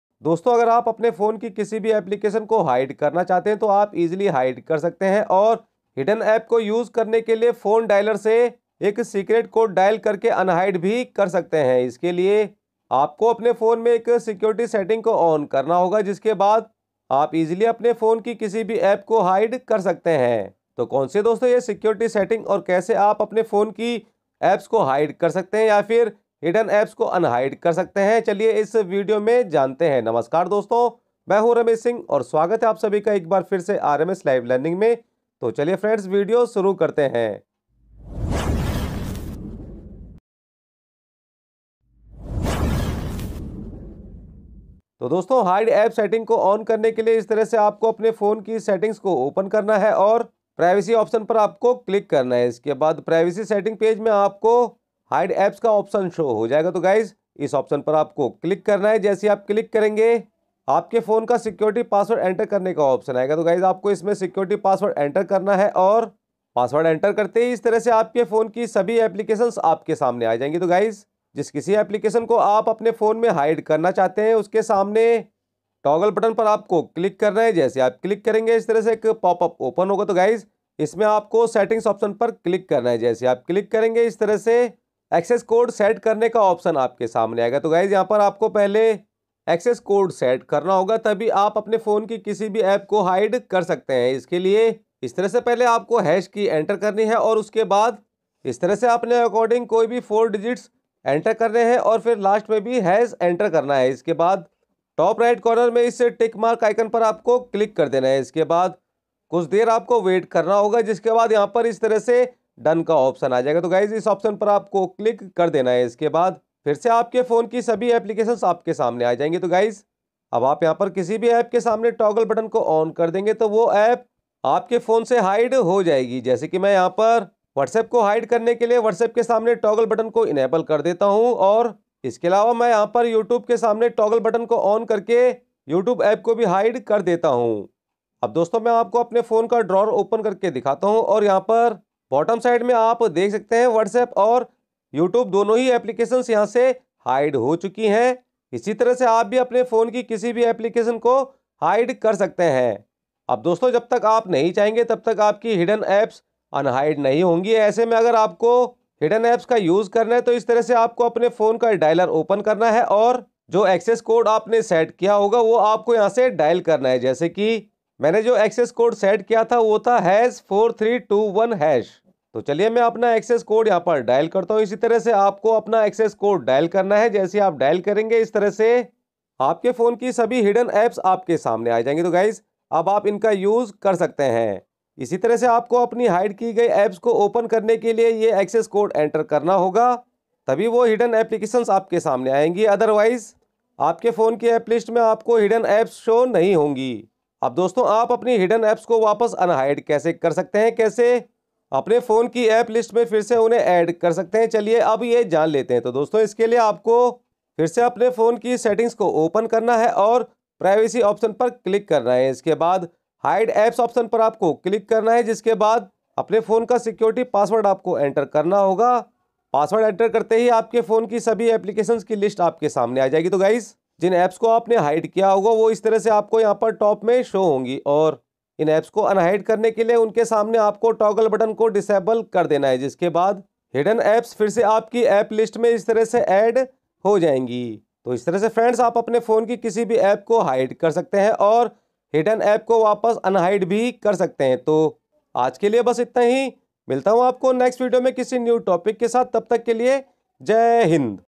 Phone me App Hide or unhide kaise kare | How to hide or unhide apps on your android mobile phone
Phone ki koi bhi app hide kaise kare or hidden apps unhide kaise kare iss video me mein aapko batane wala hu so agar aap apne mobile ki koi bhi application chhupana chahte hai to iss video me batayi secret setting ko enable karke aap easily kisi bhi app ko chupa sakte hai aur wapas hidden app ko unhide bhi kar sakte hai puri jankari ke liye ye video dekhe.